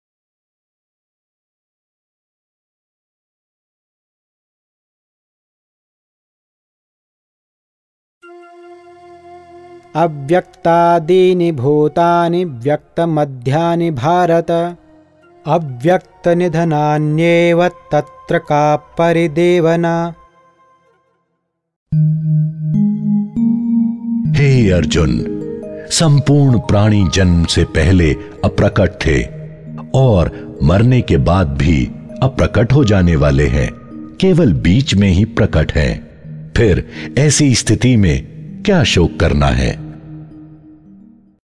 Bhagavad Gita: Chapter 02 - Shloka 28
Sanskrit recitation, followed by Hindi translation of Chapter 2, Verse 28.

Text:
अव्यक्तादीनि भूतानि व्यक्तमध्यानि भारत।
अव्यक्तनिधनान्येव तत्र का परिदेवना ।।२८।।

Translation:
हे अर्जुन! सम्पूर्ण प्राणी जन्म से पहले अप्रकट थे और मरने के बाद भी अप्रकट हो जाने वाले हैं, केवल बीच में ही प्रकट है फिर ऐसी स्थिति में क्या शोक करना है?

OVERVIEW:
The Bhagavad Gita is a 700-verse scripture that is part of the Hindu epic Mahabharata. This scripture contains a conversation between Pandava prince Arjuna and his guide Lord Krishna on a variety of theological and philosophical issues.
Faced with a fratricidal war, a despondent Arjuna turns to his charioteer Krishna for counsel on the battlefield. Krishna, through the course of the Gita, imparts to Arjuna wisdom, the path to devotion, and the doctrine of selfless action.